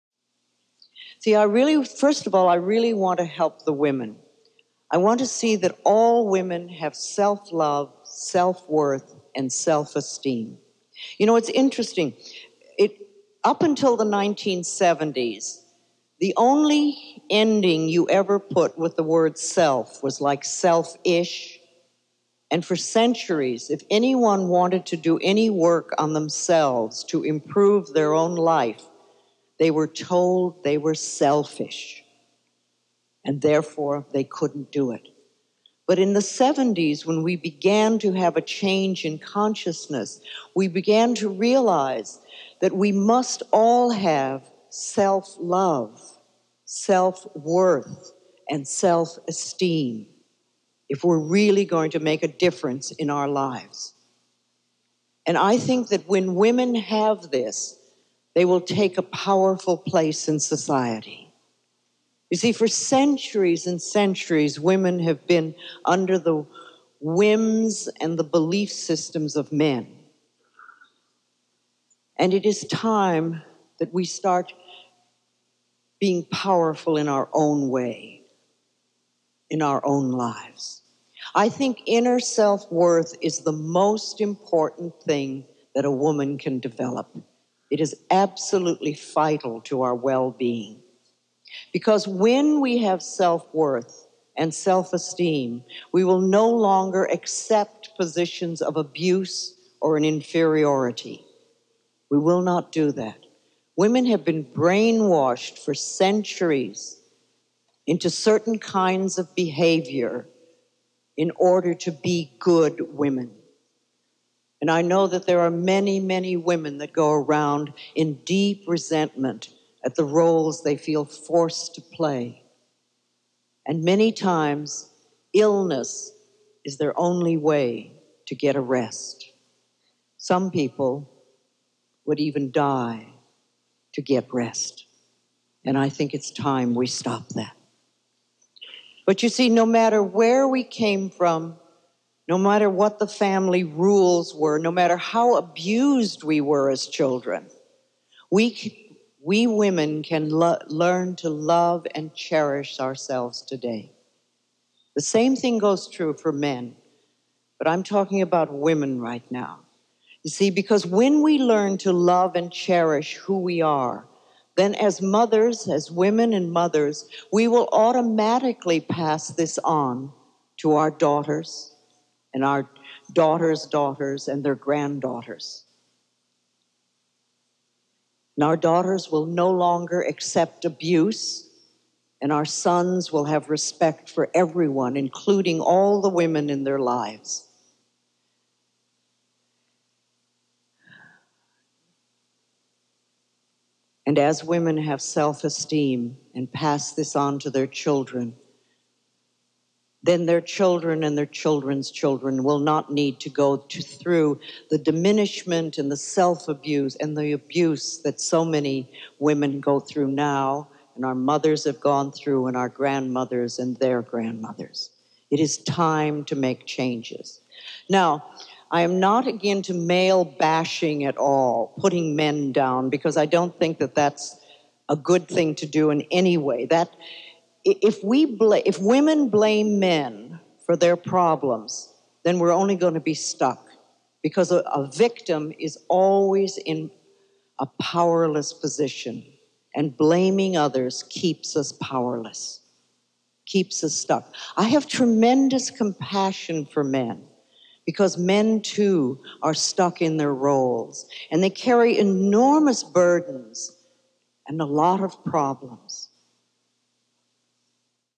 Louise Hay On Modern Feminism
Notes:

Excerpt from "Embracing Change: Using The Treasures Within You"

Remastered by Ted Clayton (me)